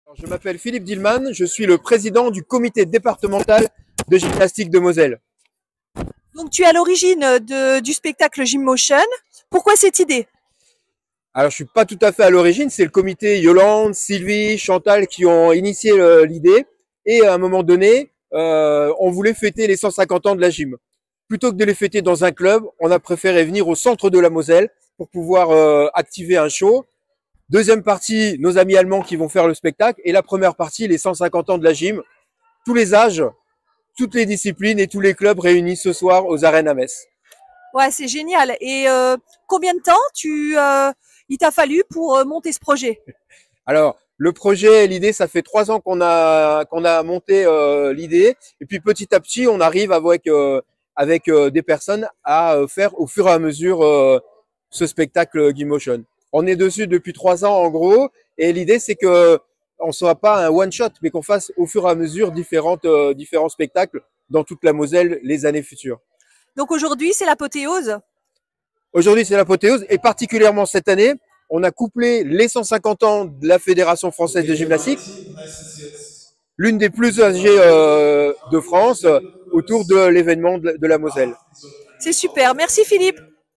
Interview Philippe Dillmann - président CD57 GYM
spectacle Gymmotion 2023